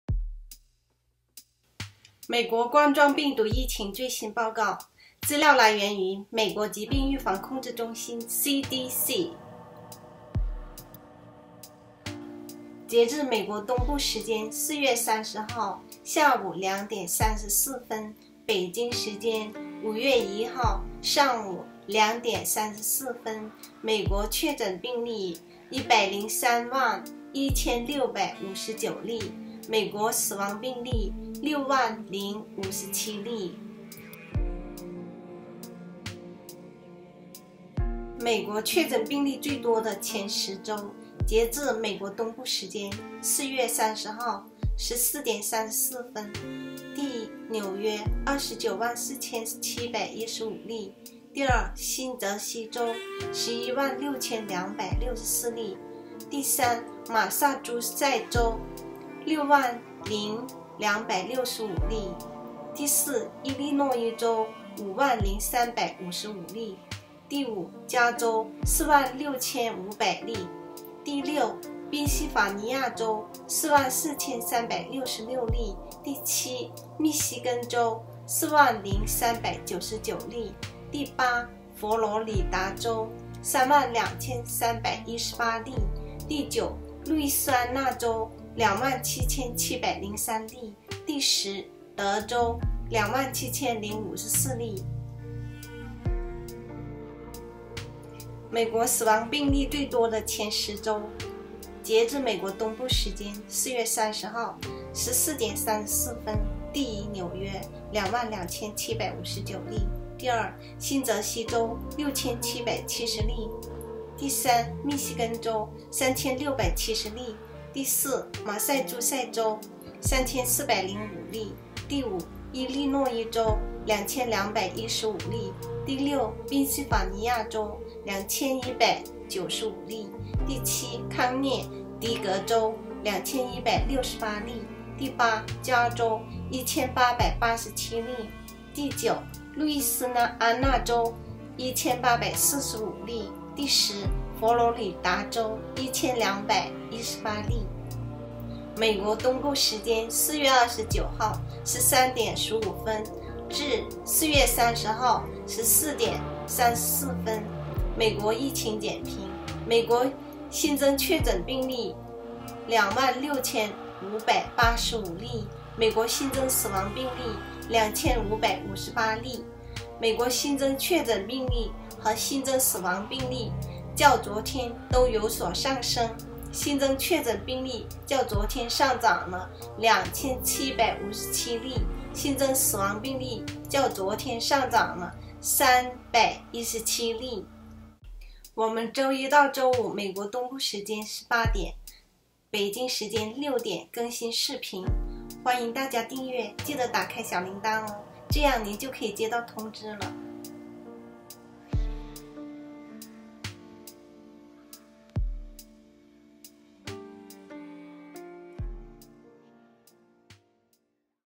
美国新冠肺炎疫情最新报告4月30号(截至美国东部时间4/30--14:34)--美国疾病预防控制中心CDC(Updates about Covid-19 in U.S) 4/40/2020
美国新冠肺炎疫情最新报告4月30号(截至美国东部时间4/30--14:34)--美国疾病预防控制中心CDC 美国新冠确诊病例，美国新冠肺炎死亡病例，新增确诊病例，新增死亡病例。(4/30/2020)
Updates about Covid-19 in U.S,  Covid-19 Cases and Deaths updated on April 30